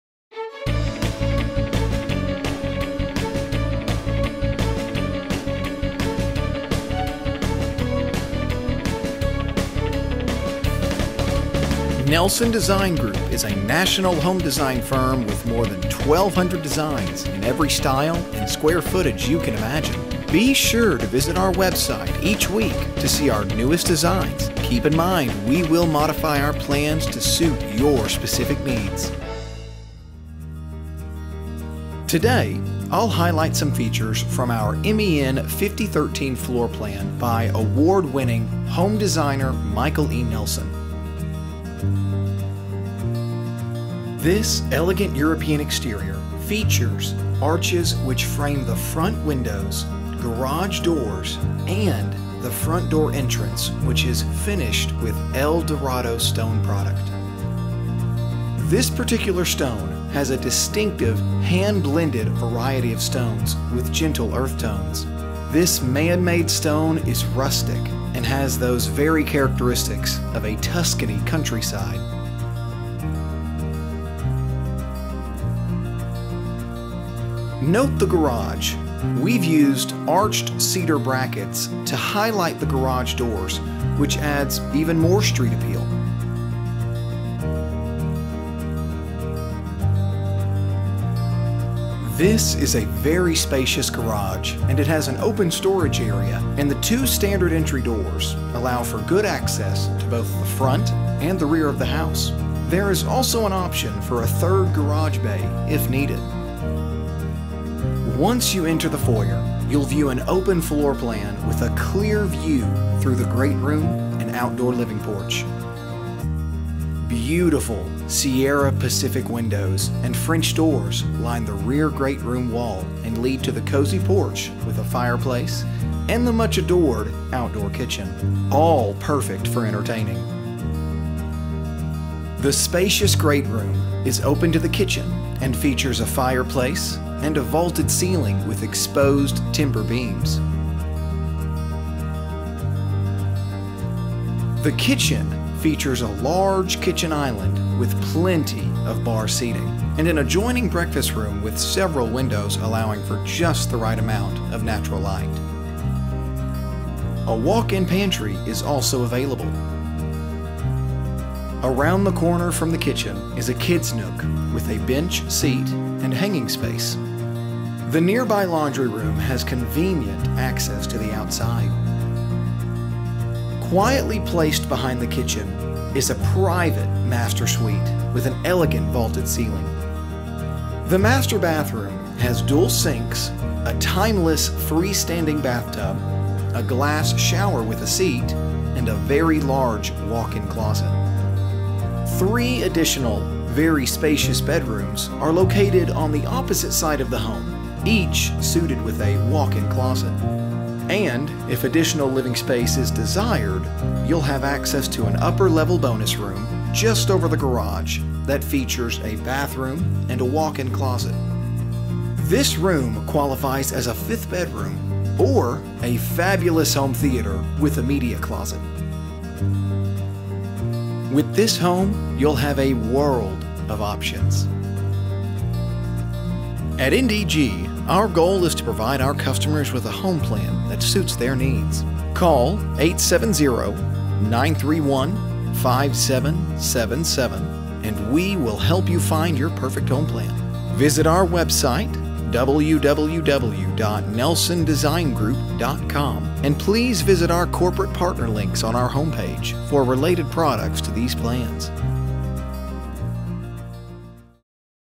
House Plan MEN 5013 (Highfield Manor) Visual Open House
MEN 5013 - Highfield Manor
2,647 Sq.Ft. | 4 Bed | 2.5 Bath

For more information on this Home Plan, visit - houseplans.ninja/content/5013-highfield-manor

This rustic home blends brick, stone, shake siding, and timber columns for a striking exterior. The foyer leads you to the Great Room with its vaulted ceiling and fireplace. The perfect area for entertaining, your guests will enjoy the curved eat-at island bar of the kitchen. And open the French Doors of the Great Room to the Outdoor Living/Grilling Porch with a cozy fireplace of its own. The Master Suite offers an optional door to the porch and a huge walk-in closet. The bathroom features double vanities, a corner whirlpool tub, and a corner glass shower with seats to ensure relaxation. On the other side of the house, you’ll find bedrooms 2, 3, and 4, each with a walk-in closet and sharing a bathroom. Upstairs there is a huge bonus room that would be perfect for another bedroom, with a full bathroom, and a large walk-in closet.

and our Corporate Partner links on our homepage for related products that compliment these plans.